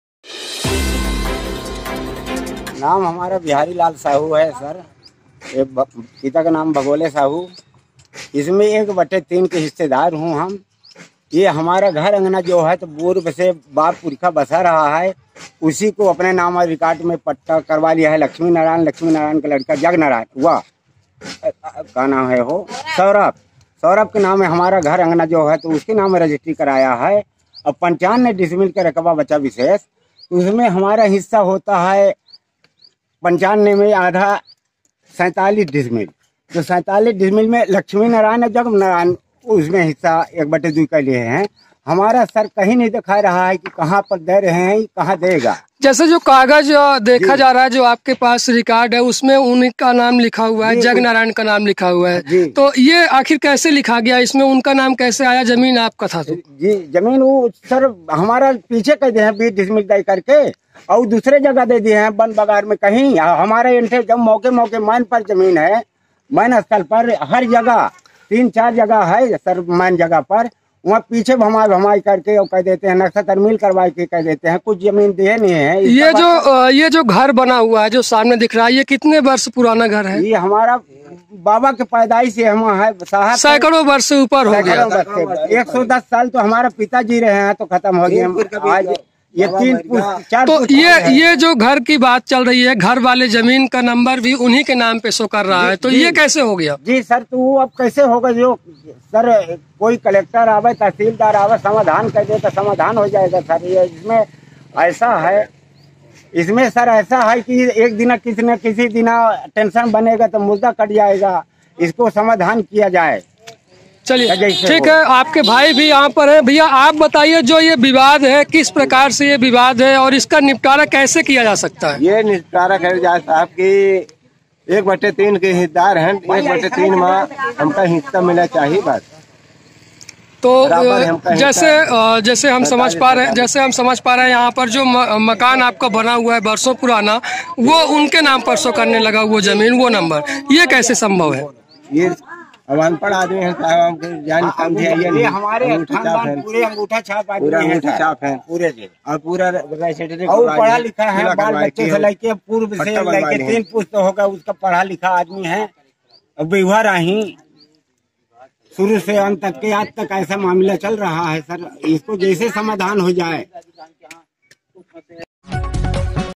सिंगरौली पक्ष-2 वर्षों से चल रहे जमीनी विवाद के कारण दोनों पक्षों में हो सकता है घमासान युद्ध
‎UD-NEWS से वीडियो